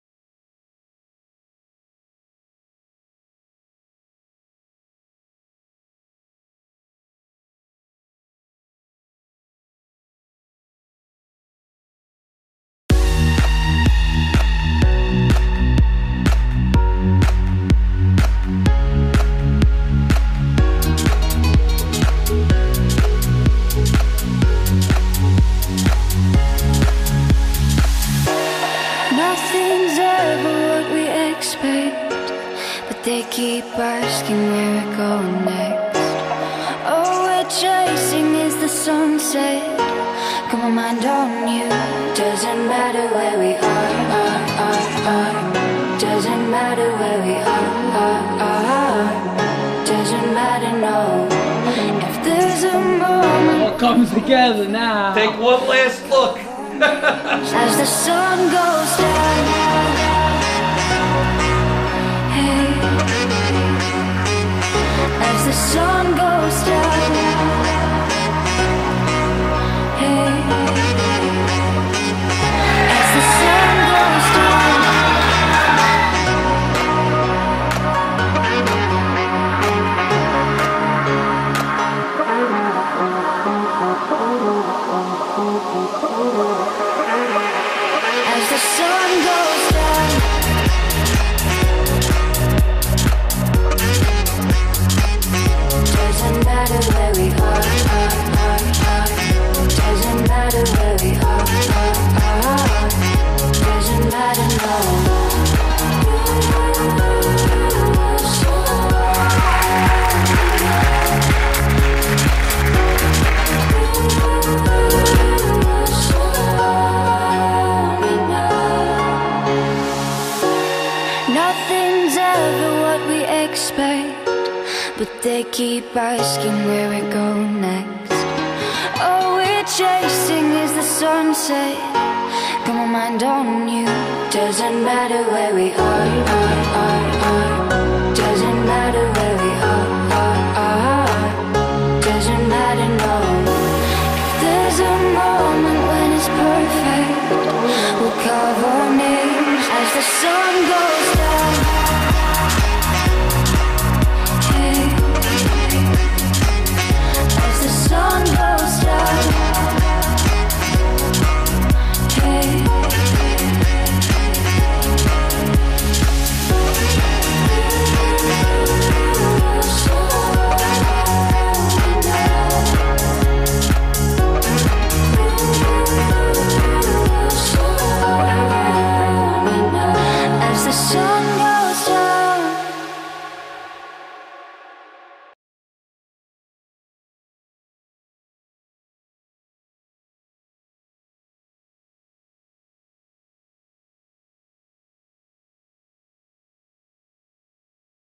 Samantha and Anthony's Trailer
Samantha and Anthony were married at the historic Oheka Castle on Long Island.  Football, Flowers and Fireworks were just part of their Amazing wedding!!